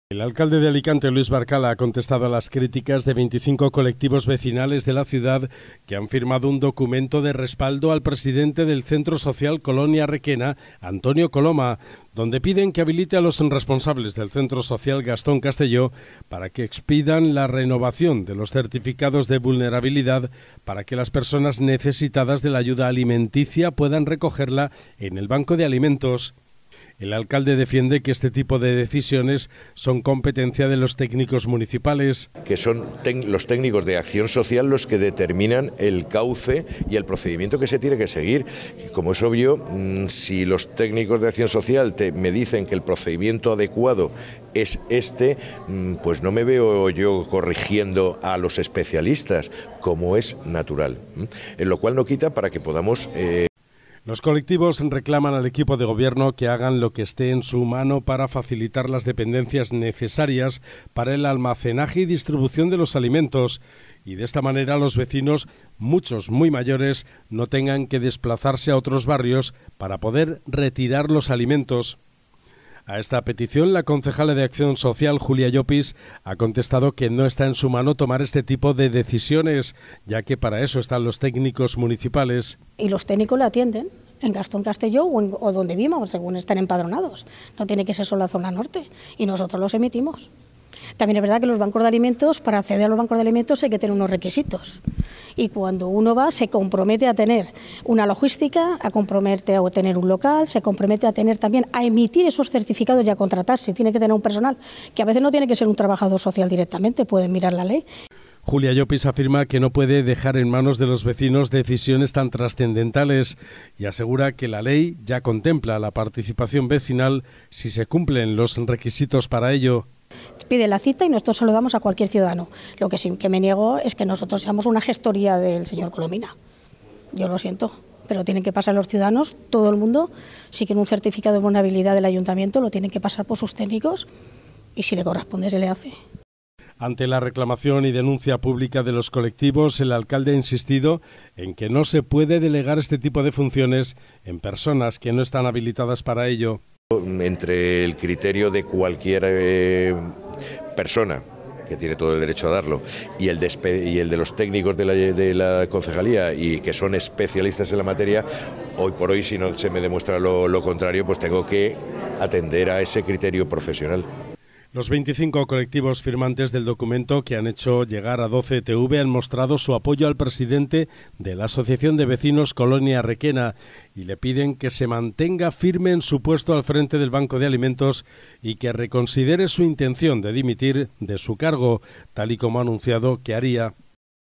El alcalde y la concejala de Acción Social contestan a los colectivos que piden competencias para
El alcalde de Alicante, Luis Barcala, ha contestado a las críticas de 25 colectivos vecinales de la ciudad que han firmado u documento en respaldo al presidente del Centro Social Colonia Requena, Antonio Coloma, donde piden que habilite a los responsables del Centro Social Gastón Castelló para que expidan la renovación de los certificados de vulnerabilidad para que las personas necesitadas de la ayuda alimenticia puedan recogerla en el Banco de Alimentos.